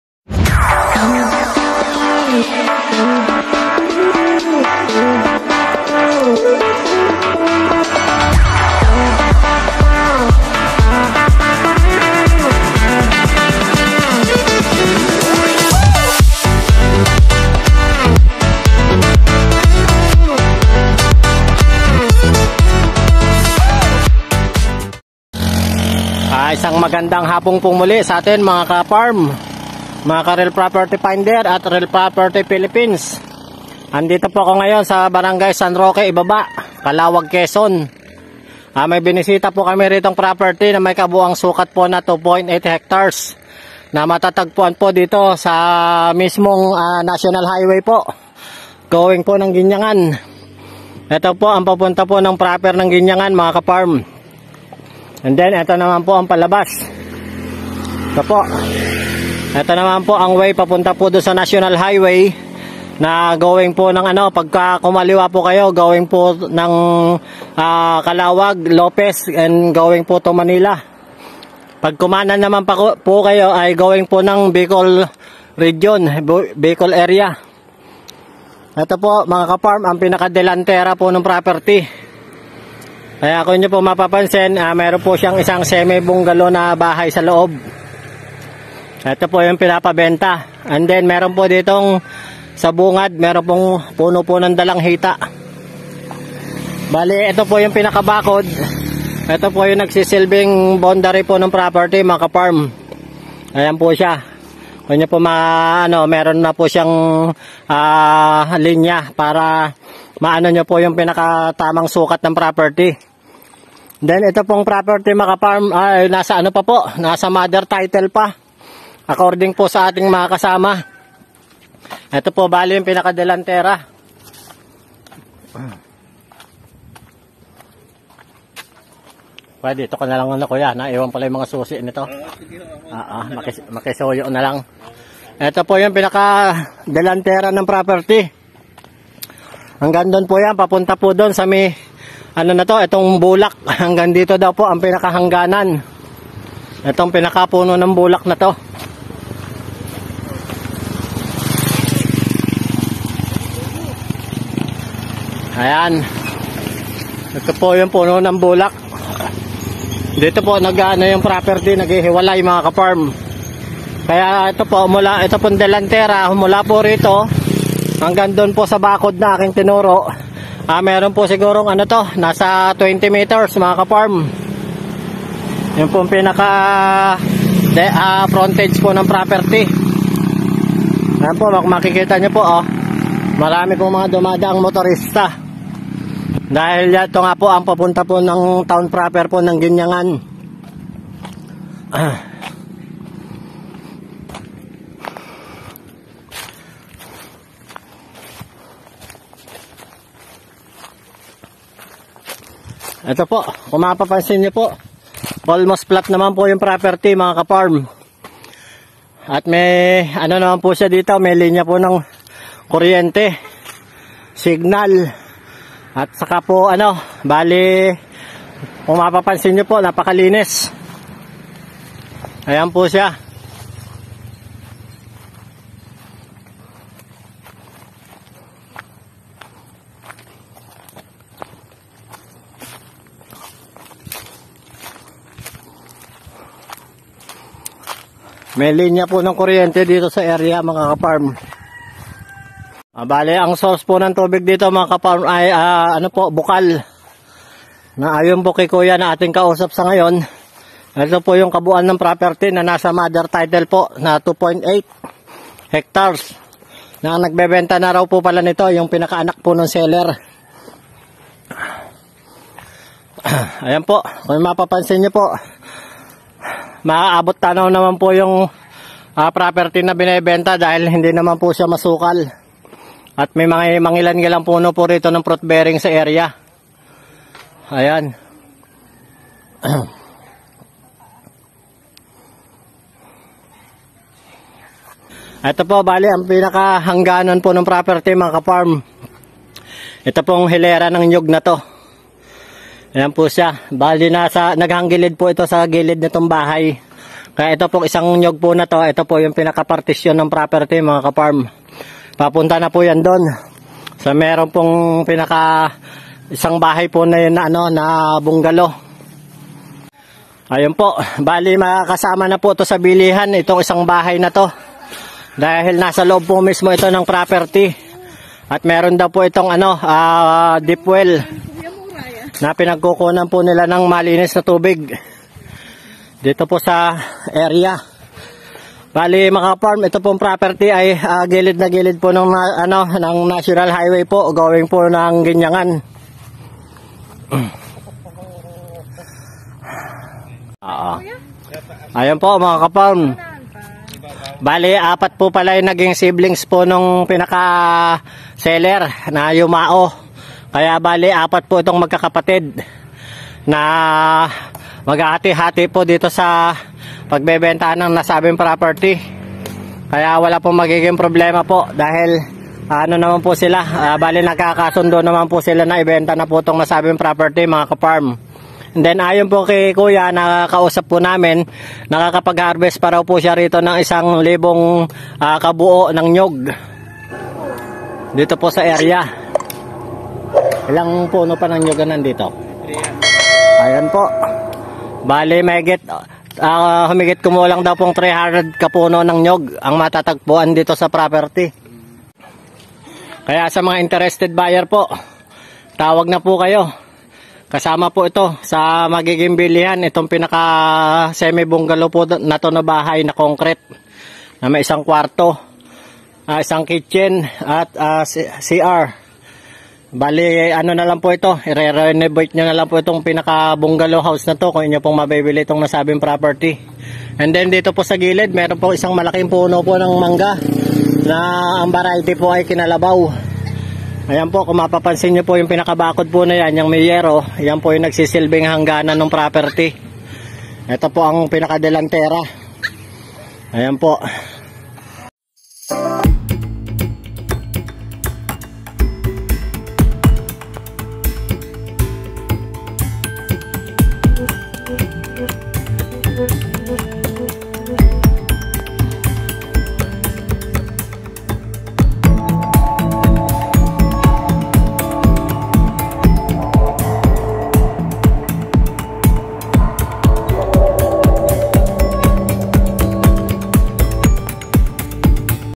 16A-329 Property Along National Highway Going to Guinyangan Quezon | With free 1 Bungalow House!!!
16A-329!
📍Location at, Calauag Quezon
📍Lot Size: 2.8 Hectares
📍Land Description: 80% Flat & 20% Semi Downhill
📍Along National Highway Going to Guinyangan Town
📍With 1 BUNGALOW House | 1Room | 1 Kitchen | 1Comfort Room
📍Source of Water: Bukal
📍With Electricity & CP Signal
📍Mother Title
📍Updated Land Tax

🌳Coconut Tree - 300
🌳Banana Tree
🌳Mango Tree
🌳Bilimbi Tree
🌳Cacao
🌳Avocado
🌳Langka
🌳Kamias

🌲Mahogany
🌲Batino
🌲Acacia